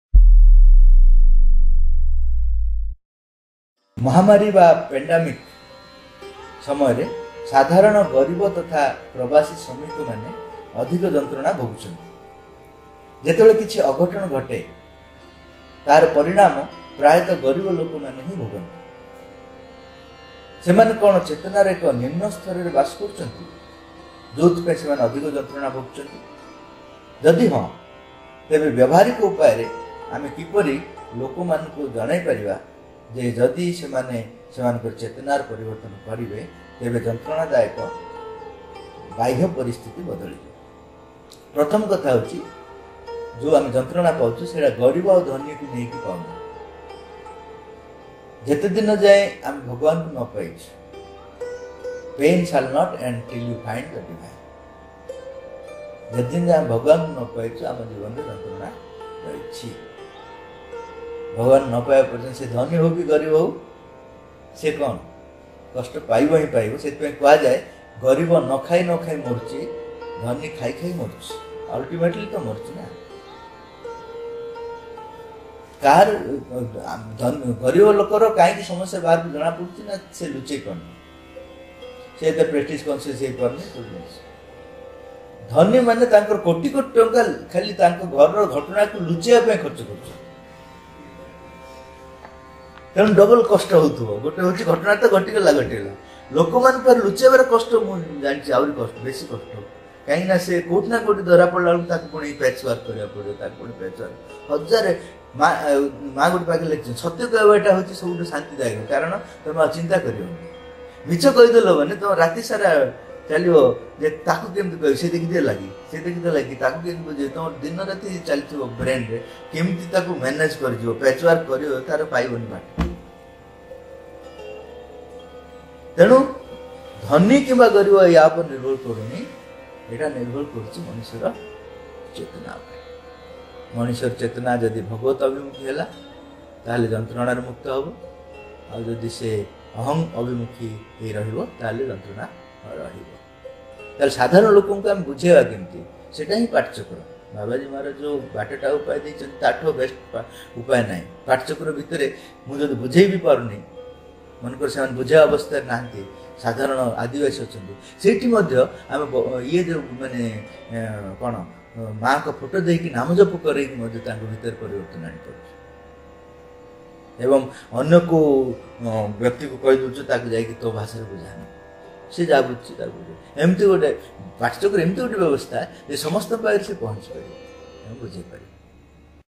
ଚେତନା ର ପରିବର୍ତ୍ତନ କରିବେ, ତେବେ ଯନ୍ତ୍ରଣାଦାୟକ ବାହ୍ୟ ପରିସ୍ଥିତି ବଦଳିଯିବ ? || Ch. Nirakar Bhai || ODIA
ପ୍ରଶ୍ନ:-
ମହାମାରୀ ବା ପେଣ୍ଡାମିକ  ସମୟରେ ସାଧାରଣ ଗରୀବ ତଥା ପ୍ରବାସୀ ଶ୍ରମିକମାନେ ଅଧିକ ଯନ୍ତ୍ରଣା
ଭୋଗୁଛନ୍ତି । ଯେତେବେଳେ କିଛି ଅଘଟଣ ଘଟେ, ତାହାର ପରିଣାମ ପ୍ରାୟତଃ ଗରିବ ଲୋକମାନେ ଭୋଗନ୍ତି କାହିଁକି ? ସେମାନେ କ'ଣ ଚେତନାର ଏକ ନିମ୍ନ ସ୍ତରରେ ବାସ କରୁଛନ୍ତି, ଯେଉଁଥିପାଇଁ ସେମାନେ ଅଧିକ ଯନ୍ତ୍ରଣା ଭୋଗୁଛନ୍ତି ? ଯଦି ହଁ, ତେବେ ବ୍ୟବହାରିକ ଉପାୟରେ ଆମେ କିପରି ଲୋକମାନଙ୍କୁ ଜଣାଇ ପାରିବା ଯେ ଯଦି ସେମାନେ ସେମାନଙ୍କର ଚେତନା ର ପରିବର୍ତ୍ତନ କରିବେ, ତେବେ ଯନ୍ତ୍ରଣାଦାୟକ ବାହ୍ୟ ପରିସ୍ଥିତି ବଦଳିଯିବ ?